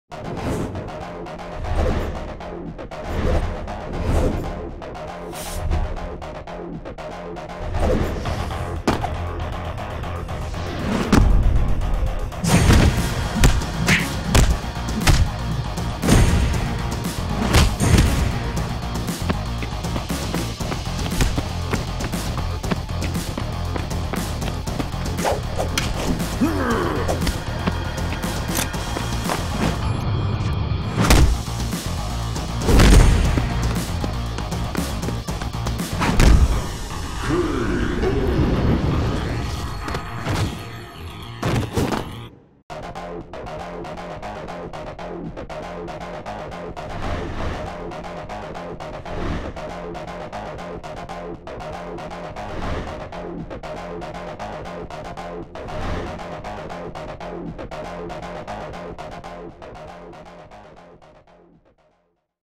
Red Bolt vs. Green Fist - Vancouver Film School (VFS)
It's time for the ultimate fight 💥 Red Bolt meets Green Fist in this intense clip created by VFS 3D Animation program grad Julian Coronado Labrada. What team are you on? Let us know in comments!